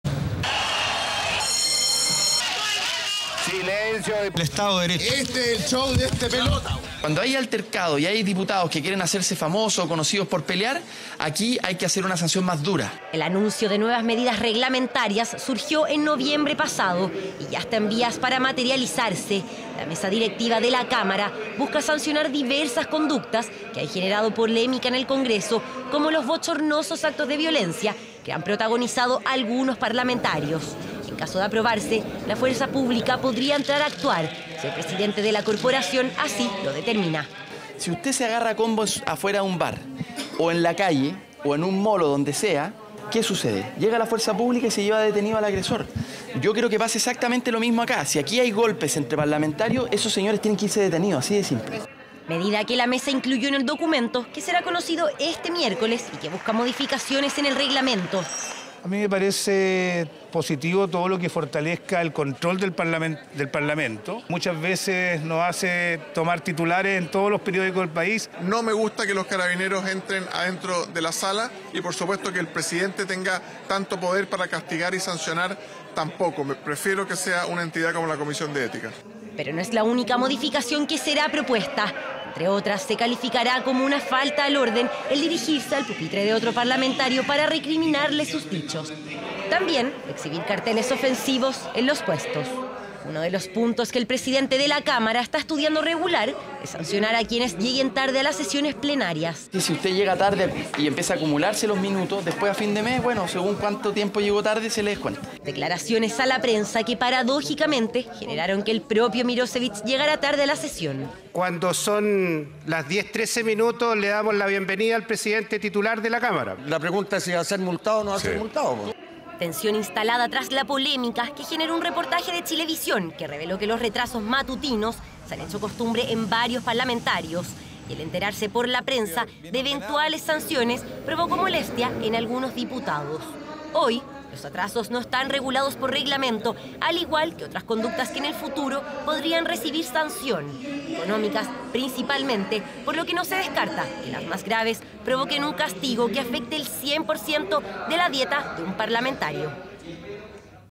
Agresiones, ofensas y retrasos: Cámara busca endurecer sanciones
Los atrasos, el exhibir carteles ofensivos y las agresiones verbales y físicas podrían tener nuevas y severas sanciones si prospera la idea de la mesa de la Cámara de Diputadas y Diputados que busca modificar el actual reglamento interno.